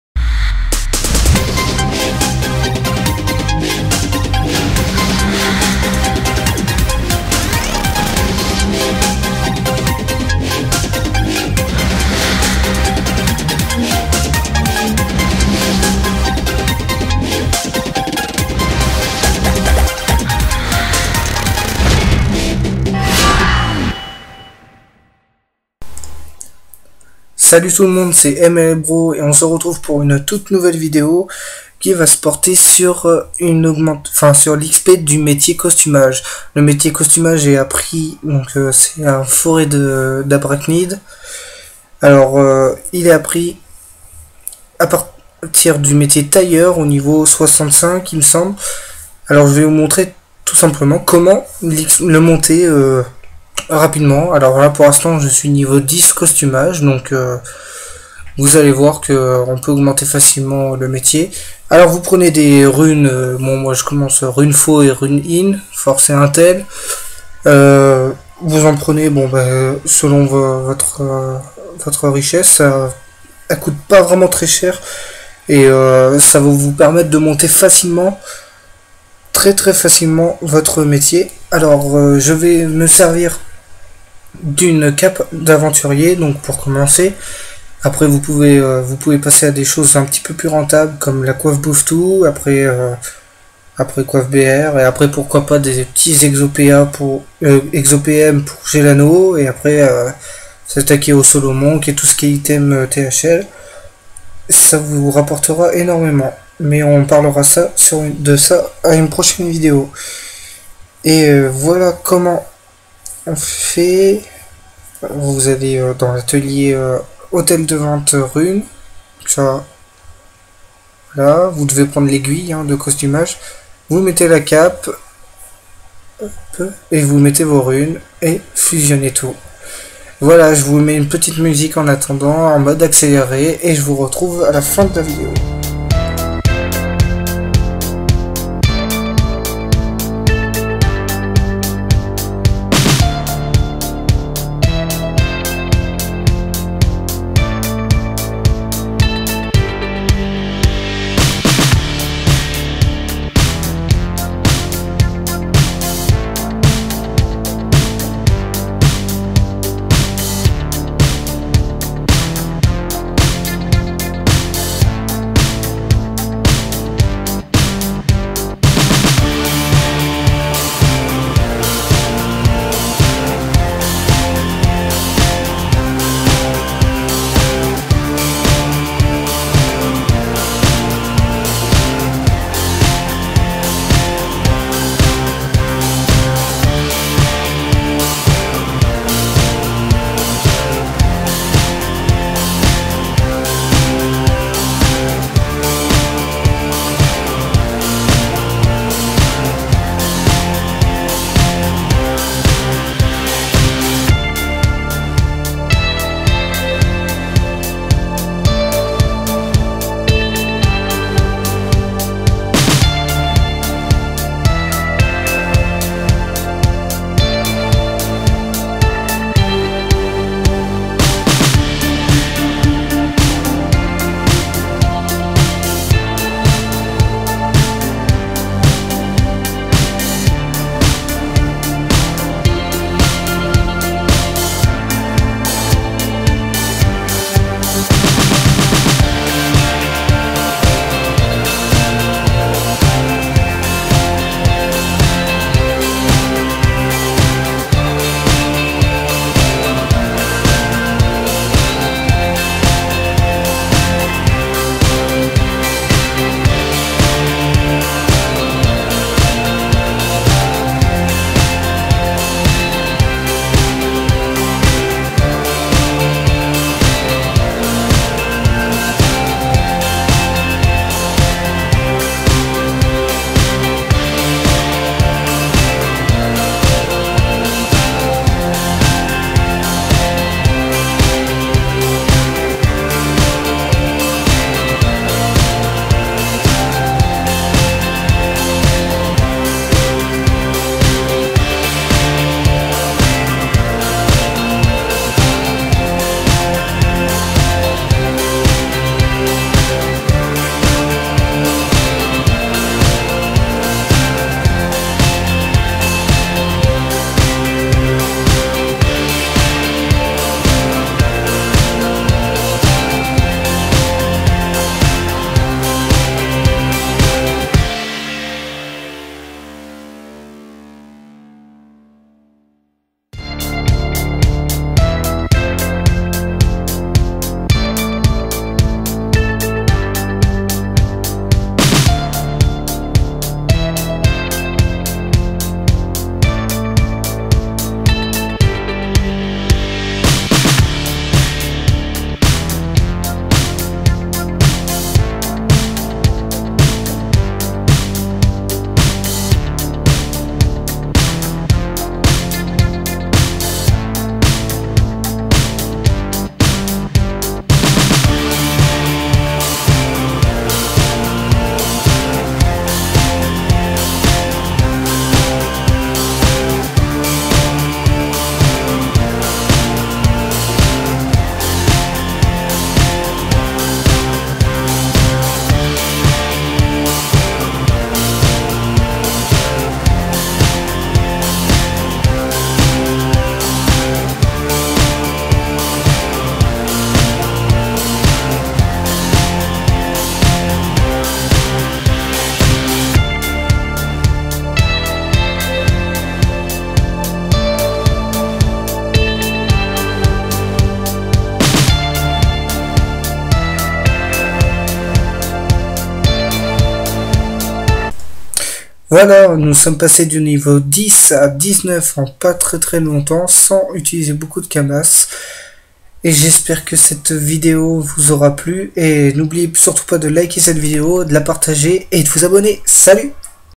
MetL Bro -- Comment monter rapidement le métier costumage
Je vous montre cette toute nouvelle vidéo pour vous aidez a monter votre métier costumage, bien sur cette vidéo fait part a une suite pour un petit tuto kamas que je me presse a vous montrer, celle-ci est encore en étude, bon visionnage a tous =).